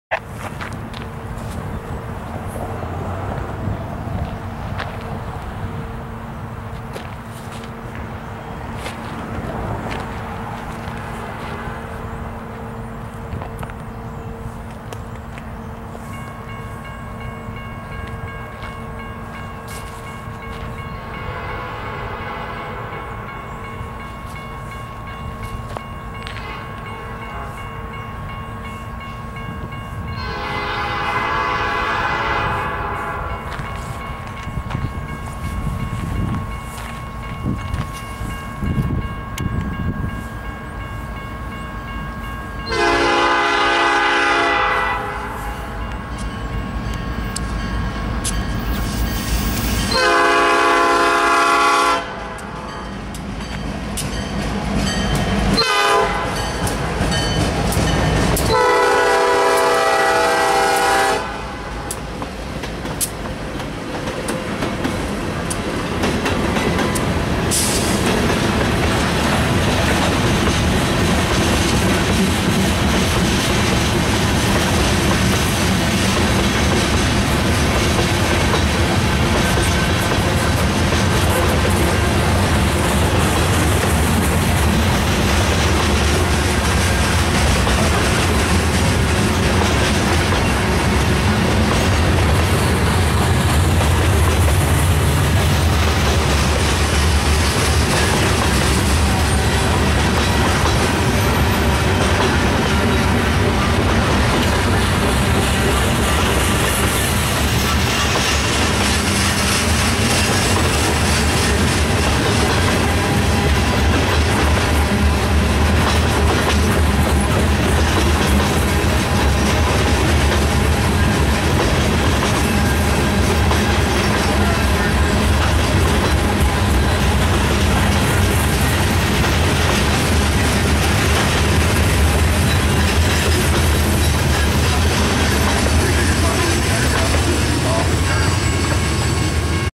GTW 5853 Neenah, WI 11-17-12
Southbound local:
GTW 5853 GP38-2
EJ&E 663 SD38-3

CN Neenah Sub